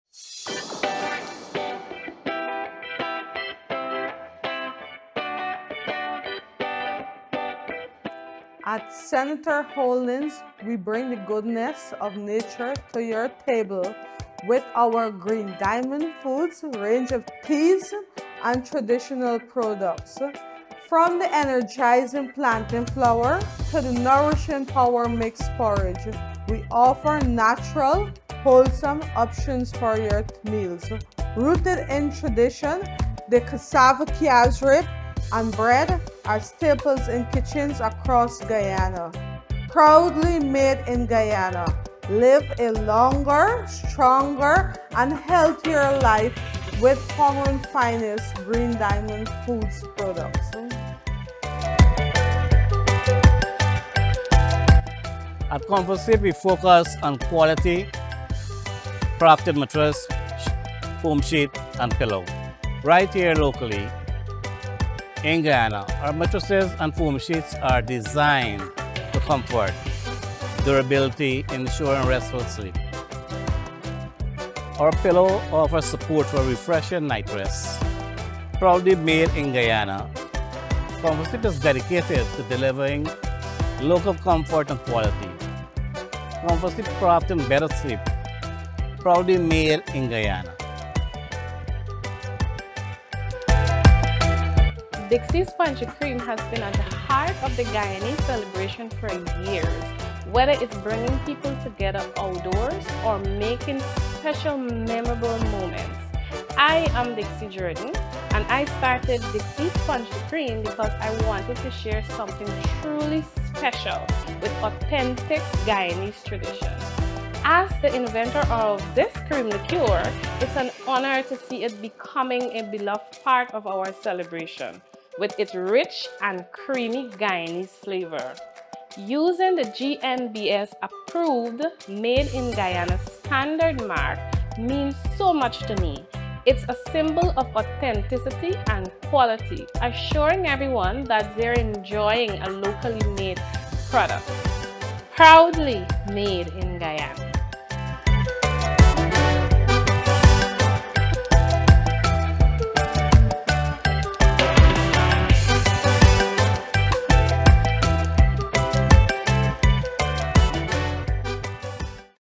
We’re wrapping up our series with a powerful trio of Made in Guyana GNBS Approved certifiedcompanies
We’re wrapping up our series with a powerful trio of Made in Guyana GNBS Approved certified companies that are taking the local market by storm! 🇬🇾
These businesses are not just brands, they're a movement, showcasing the best of Guyanese innovation, passion, and creativity.
With the Made in Guyana GNBS Approved mark as their seal of excellence, they’re paving the way for a brighter future for all of us. Let’s stand behind these trailblazers as they continue to shape our communities and inspire us all to go local!
Green Diamond Foods
Comfort Sleep
Dixie'S punch de cream